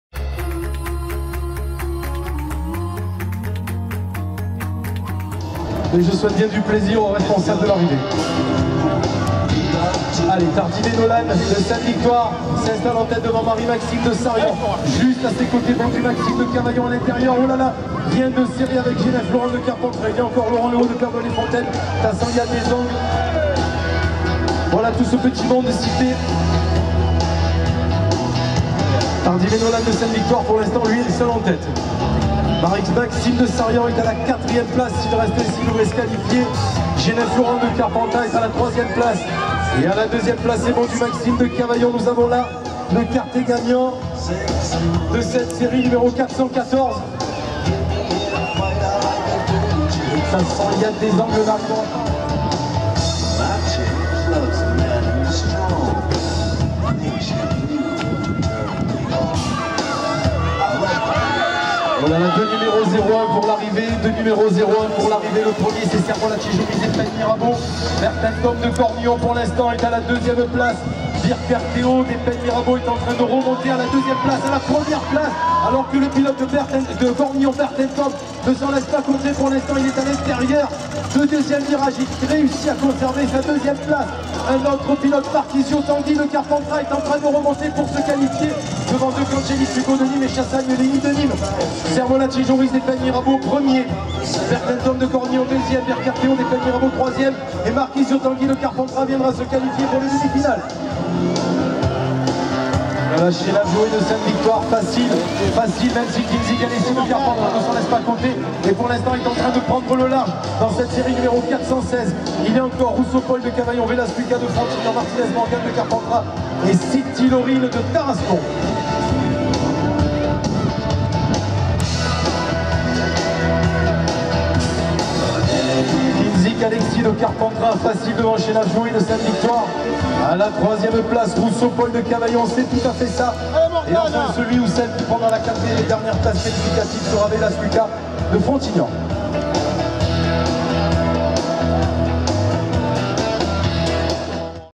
2012-05-27 Bmx Coupe de Provence Vitrolles Benjamin - Quart Finale.mp4
BMX Race à Vitrolles le 27/05/2012
7ème manche (FINALE) de la Coupe de Provence
1/4 Finale Benjamin